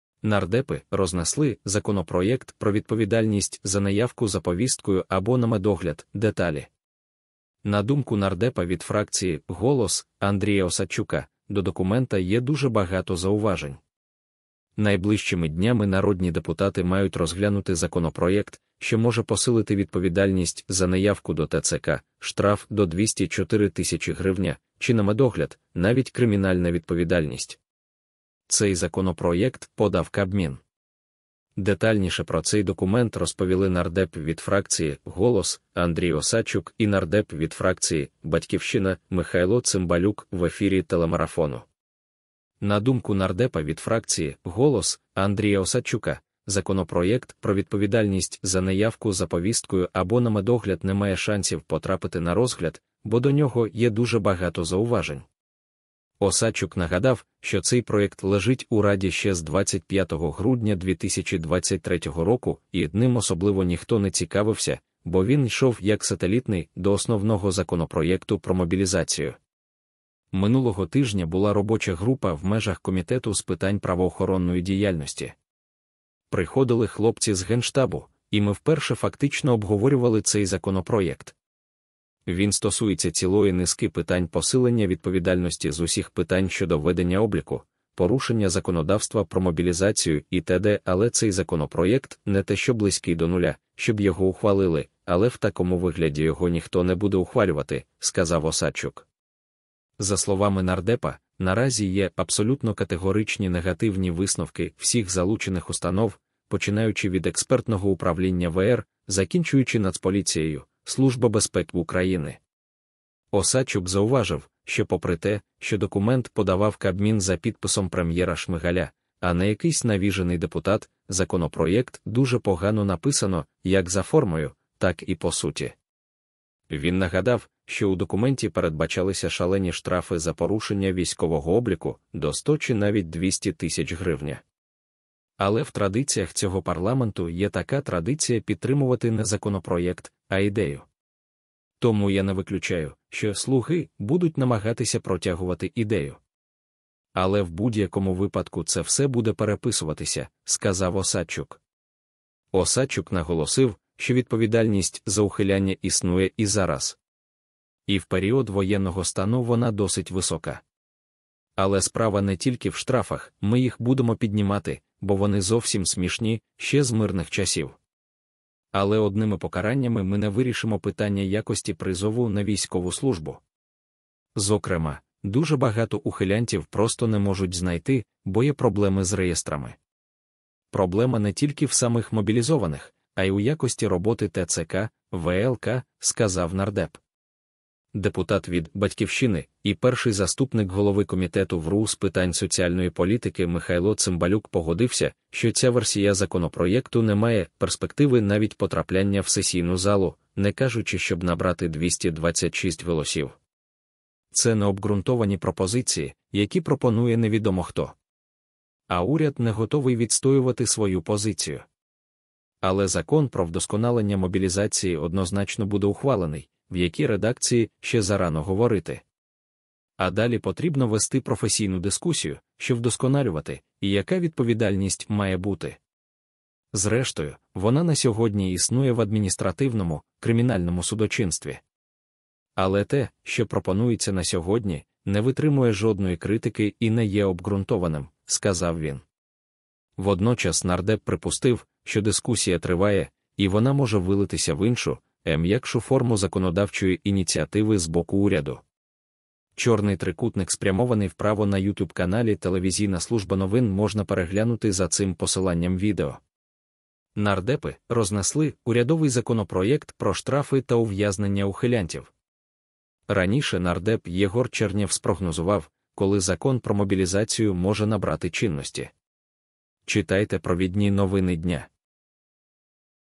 Нардепи "рознесли" законопроєкт про відповідальність за неявку за повісткою або на медогляд – деталі
На думку нардепа від фракції "Голос" Андрія Осадчука, до документа є дуже багато зауважень.

Найближчими днями народні депутати мають розглянути законопроєкт, що може посилити відповідальність за неявку до ТЦК (штраф до 204 тис. грн) чи на медогляд (навіть кримінальна відповідальність). Цей законопроєкт подав Кабмін.

Детальніше про цей документ розповіли нардеп від фракції "Голос" Андрій Осадчук і нардеп від фракції "Батьківщина" Михайло Цимбалюк в ефірі телемарафону.

На думку нардепа від фракції "Голос" Андрія Осадчука, законопроєкт про відповідальність за неявку за повісткою або на медогляд не має шансів потрапити на розгляд, бо до нього є дуже багато зауважень.

Осадчук нагадав, що цей проєкт лежить у Раді ще з 25 грудня 2023 року і "ним особливо ніхто не цікавився, бо він йшов як сателітний" до основного законопроєкту про мобілізацію.

"Минулого тижня була робоча група в межах комітету з питань правоохоронної діяльності. Приходили хлопці з Генштабу, і ми вперше фактично обговорювали цей законопроєкт. Він стосується цілої низки питань посилення відповідальності з усіх питань щодо ведення обліку, порушення законодавства про мобілізацію і т.д. Але цей законопроєкт не те що близький до нуля, щоб його ухвалили, але в такому вигляді його ніхто не буде ухвалювати", – сказав Осадчук.

За словами нардепа, наразі є "абсолютно категоричні негативні висновки" всіх залучених установ, починаючи від експертного управління ВР, закінчуючи Нацполіцією, СБУ.

Він нагадав, що у документі передбачалися шалені штрафи за порушення військового обліку – до 100 чи навіть 200 тисяч грн.

"Але в традиціях цього парламенту є така традиція підтримувати не законопроєкт, а ідею. Тому я не виключаю, що “слуги” будуть намагатися протягувати ідею. Але в будь-якому випадку це все буде переписуватися", – сказав Осадчук.

Осадчук наголосив, що відповідальність за ухиляння існує і зараз. І в період воєнного стану вона досить висока.

"Але справа не тільки в штрафах – ми їх будемо піднімати, бо вони зовсім смішні, ще з мирних часів. Але одними покараннями ми не вирішимо питання якості призову на військову службу. Зокрема, дуже багато ухилянтів просто не можуть знайти, бо є проблеми з реєстрами. Проблема не тільки в самих мобілізованих, а й у якості роботи ТЦК, ВЛК", – сказав нардеп.

Депутат від "Батьківщини" і перший заступник голови Комітету ВРУ з питань соціальної політики Михайло Цимбалюк погодився, що ця версія законопроєкту не має "перспективи навіть потрапляння в сесійну залу, не кажучи щоб набрати 226 голосів".

"Це необґрунтовані пропозиції, які пропонує невідомо хто. А уряд не готовий відстоювати свою позицію. Але закон про вдосконалення мобілізації однозначно буде ухвалений, в якій редакції – ще зарано говорити. А далі потрібно вести професійну дискусію, що вдосконалювати, і яка відповідальність має бути. Зрештою, вона на сьогодні існує в адміністративному, кримінальному судочинстві. Але те, що пропонується на сьогодні, не витримує жодної критики і не є обґрунтованим", – сказав він.

Водночас нардеп припустив, що дискусія триває, і вона може вилитися в іншу, м'якшу форму законодавчої ініціативи з боку уряду.

▶ На YouTube-каналі ТСН можна переглянути за цим посиланням відео:

Нардепи "рознесли" УРЯДОВИЙ законопроєкт про штрафи та ув'язнення ухилянтів!

Раніше нардеп Єгор Чернєв спрогнозував, коли закон про мобілізацію може набрати чинності.

Читайте провідні новини дня: